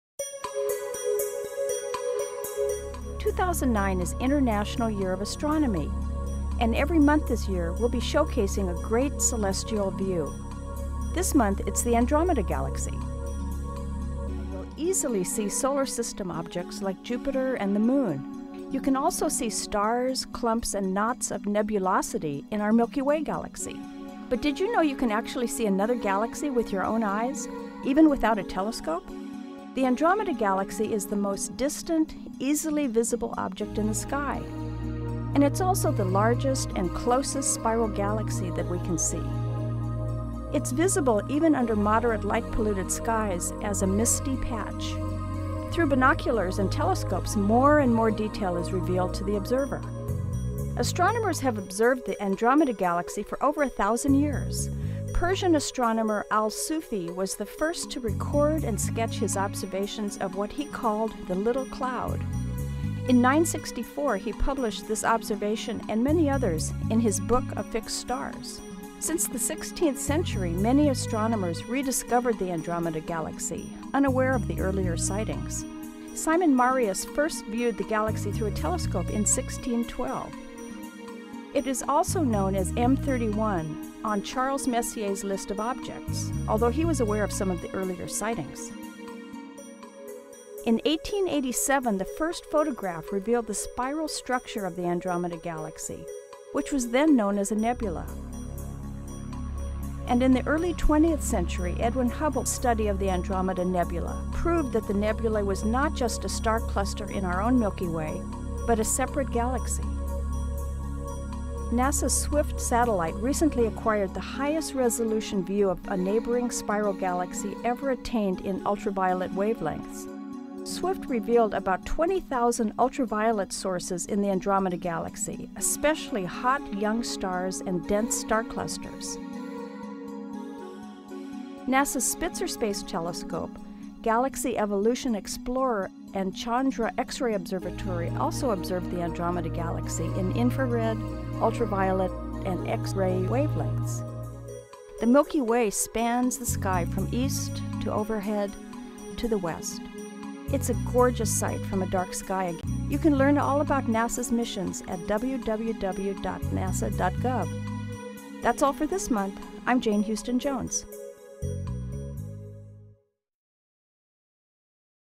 The Andromeda Galaxy
... "The Andromeda Galaxy" with Jane Houston Jones at NASA's Jet Propulsion Laboratory in Pasadena, California.

The Andromeda Galaxy (also known as Messier 31, M31, or NGC 224) is a spiral galaxy approximately 2,500,000 light-years away in the constellation Andromeda. It is the nearest spiral galaxy to our own, the Milky Way. As it is visible as a faint smudge on a moonless night, it is one of the farthest objects visible to the naked eye, and can be seen even from urban areas with binoculars.

It is named after the princess Andromeda (Greek: Ανδρομέδη  Andromédē) in Greek mythology. Andromeda is the largest galaxy of the Local Group, which consists of the Andromeda Galaxy, the Milky Way Galaxy, the Triangulum Galaxy, and about 30 other smaller galaxies.

Although the largest, Andromeda may not be the most massive, as recent findings suggest that the Milky Way contains more dark matter and may be the most massive in the grouping.

The 2006 observations by the Spitzer Space Telescope revealed that M31 contains one trillion stars, several times more than the number of stars in our own galaxy, which is estimated to be c. 200-400 billion.

While the 2006 estimates put the mass of the Milky Way to be ~80% of the mass of Andromeda, which is estimated to be 7.1 × 1011 solar masses, a 2009 study concluded that Andromeda and the Milky Way are about equal in mass.

At an apparent magnitude of 3.4, the Andromeda Galaxy is notable for being one of the brightest Messier objects, making it easily visible to the naked eye even when viewed from areas with moderate light pollution.

Although it appears more than six times as wide as the full moon when photographed through a larger telescope, only the brighter central region is visible with the naked eye.

When you look up at the sky youll easily see solar system objects like Jupiter and the moon. You can also see stars, clumps and knots of nebulosity in our Milky Way Galaxy. But did you know you can actually see another galaxy with your own eyes, even without a telescope?

The Andromeda Galaxy is the most distant easily visible object in the sky, and its also the largest and closest spiral galaxy that we can see. Its visible even under moderate light polluted skies as a misty patch. Through binoculars and telescopes more and more detail is revealed to the observer.

Astronomers have observed the Andromeda galaxy for over a thousand years. Persian astronomer Al-Sufi was the first to record and sketch his observations of what he called the little cloud. In 964 he published this observation and many others in his Book of Fixed Stars.

Since the 16th century many astronomers rediscovered the Andromeda Galaxy unaware of the earlier sightings. Simon Marius first viewed the galaxy through a telescope in 1612.

It is also known as M-31 on Charles Messiers list of objects, although he was aware of some earlier sightings. In 1887 the first photograph revealed the spiral structure of the Andromeda Galaxy, which was then known as a nebula. And in the early 20th century Edwin Hubbles study of Andromeda nebula proved the nebula was not just a star cluster in our own Milky Way, but a separate galaxy.

NASA's Swift satellite recently acquired the highest-resolution view of a neighboring spiral galaxy ever attained in ultraviolet wavelengths. Swift revealed about 20,000 ultraviolet sources in the Andromeda Galaxy, especially hot, young stars and dense star clusters. NASAs Spitzer Space Telescope, Galaxy Evolution Explorer and Chandra X-Ray Observatory also observed the Andromeda Galaxy in infrared, ultraviolet and X-ray wavelengths.